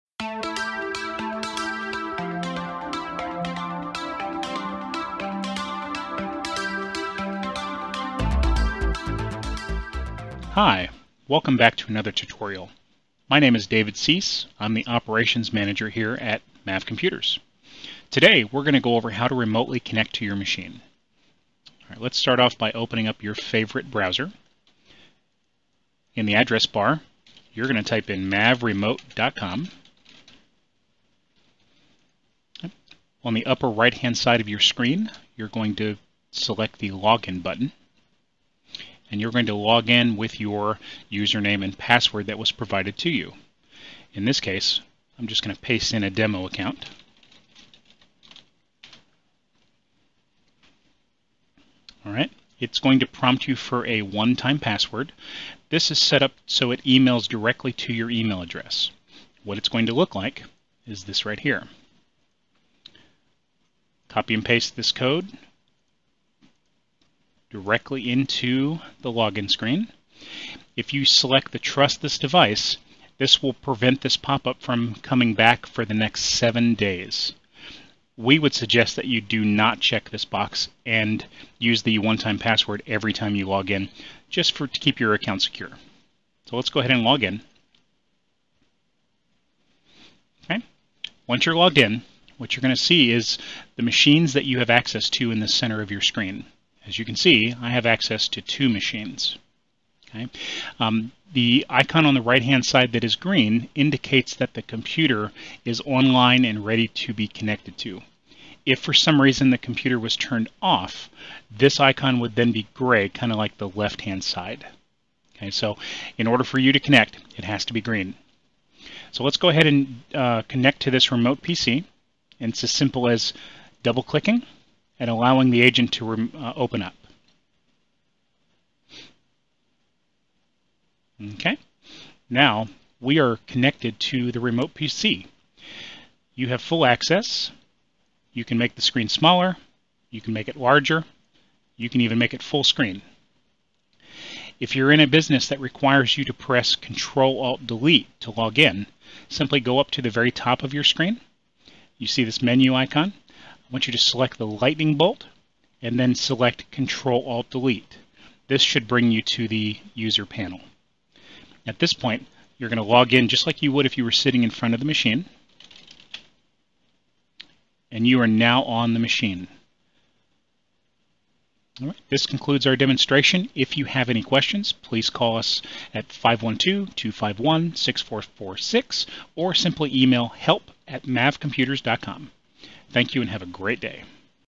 Remote Access Tutorial for MAV Clients
This tutorial goes over the basics of logging in to your machine/s remotely. You must be a MAV client or use Connectwise Control to follow along.